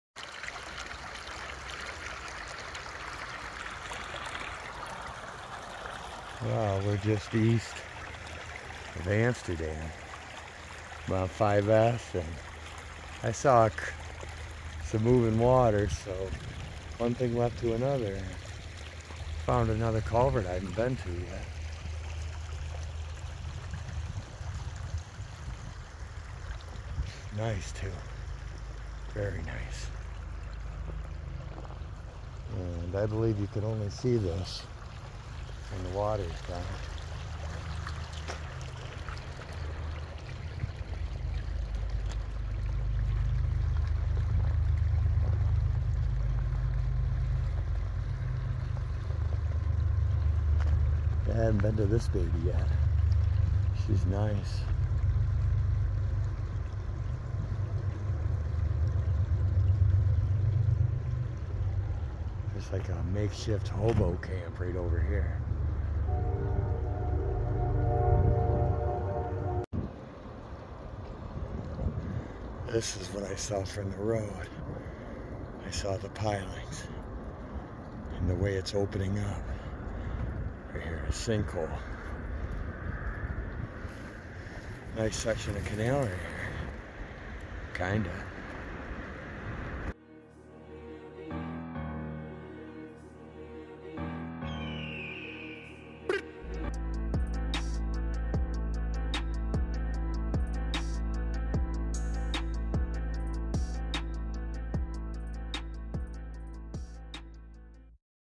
Canal Trippin & found a new culvert east of Amsterdam
We drove down the old towpath to get to this one. a little sketchy getting down the bank